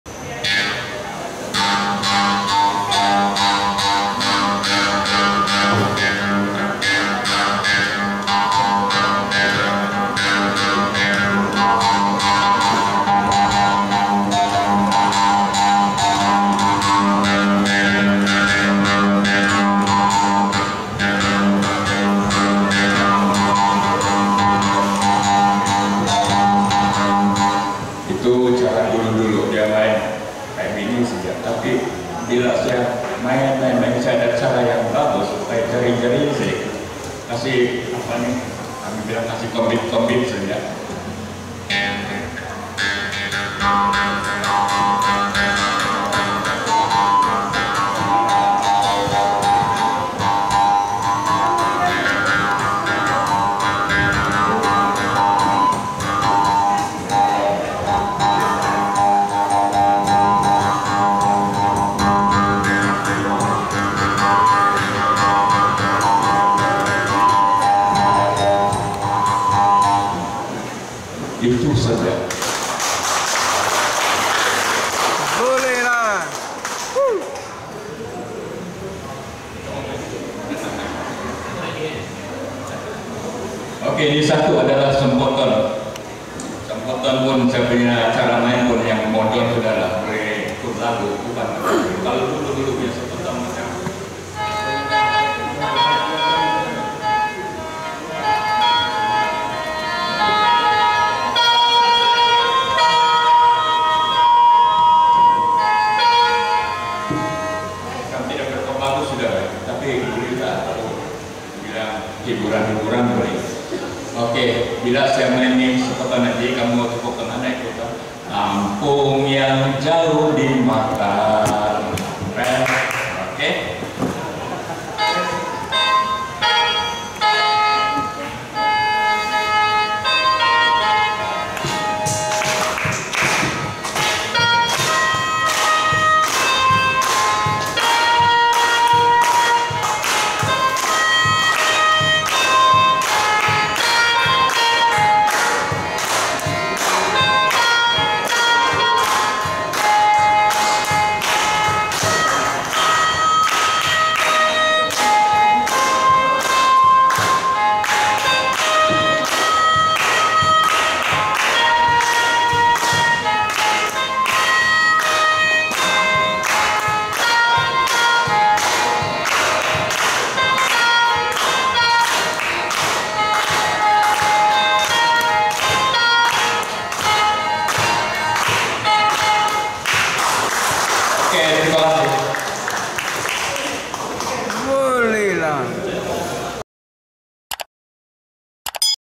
BUNGKAU AND SOMPOTON by TALENTED VILLAGE CHIEF POTUKI KAAMATAN 27 MAY 2023
LUCKY VILLAGE WHO GOT A CHIEF WHO CAN PLAY TRADITIONAL INSTRUMENTS AND SINGS AS WELL